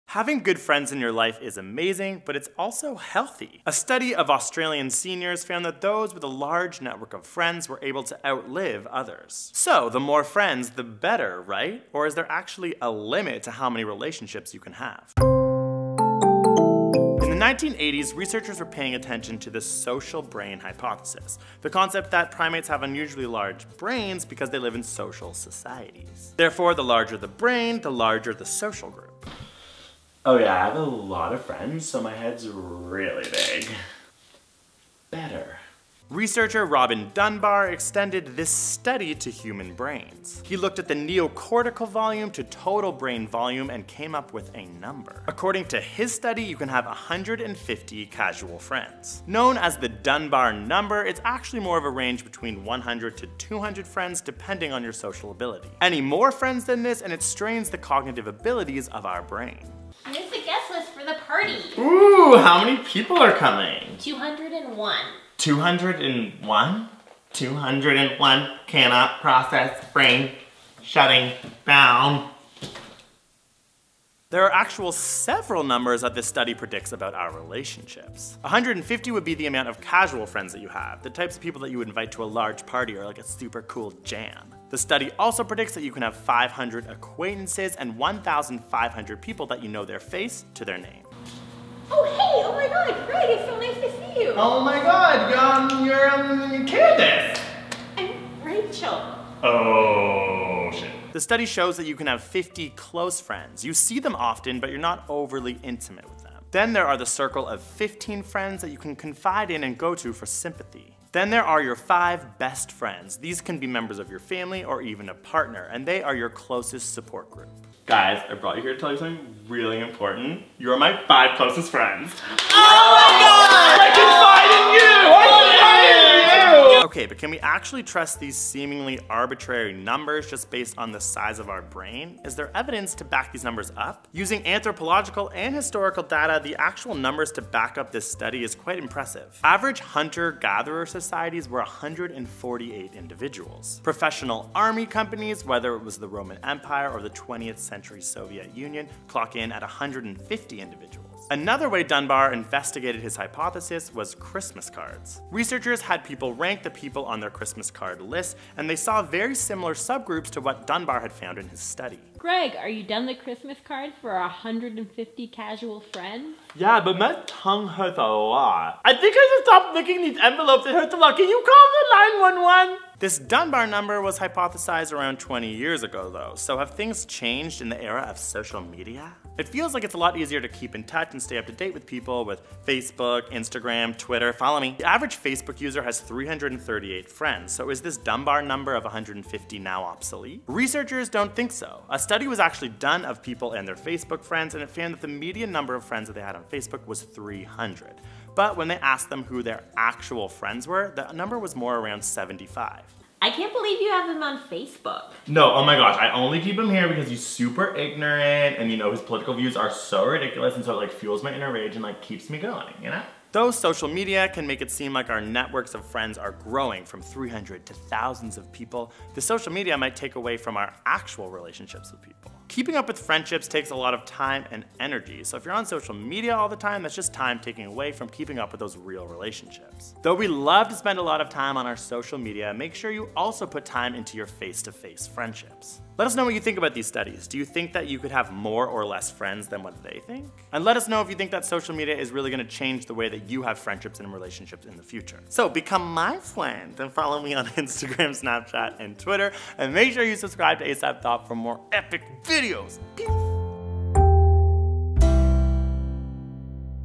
How Many Friends Can You Actually Have?
Is there a limit on how many you can have?
Follow on Instagram, Twitter & Snapchat: @whalewatchmeplz

Written by Rachel Salt and Gregory Brown

ASAPSCIENCE INC.

References:
Effect of social networks on 10 year survival in very old Australians: the Australian longitudinal study of aging